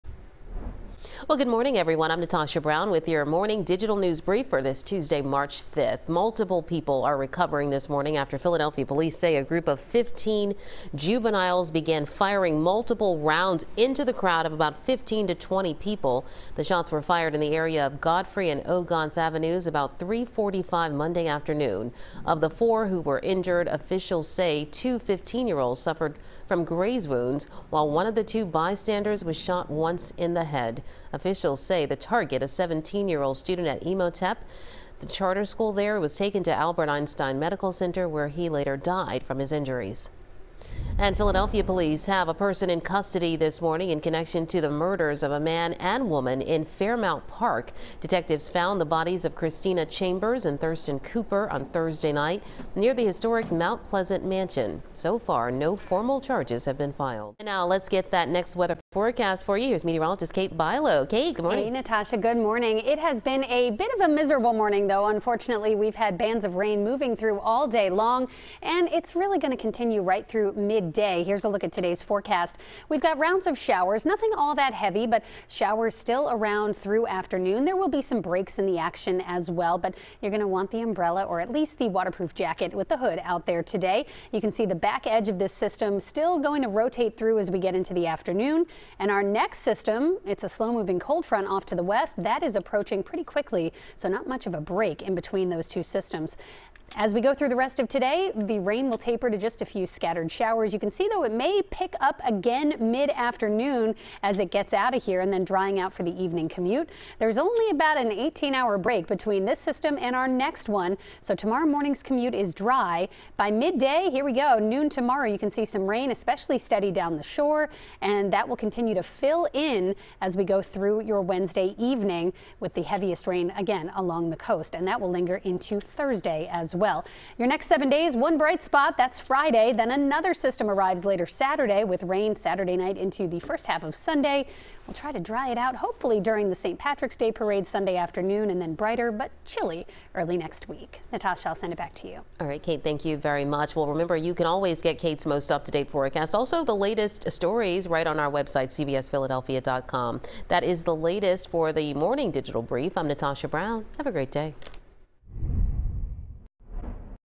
Digital Brief: March 5, 2024
A shooting at a SEPTA bus stop left a 17-year-old injured while police have a suspect in custody for a double homicide in Fairmount Park. Natasha Brown has your latest Philadelphia news headlines.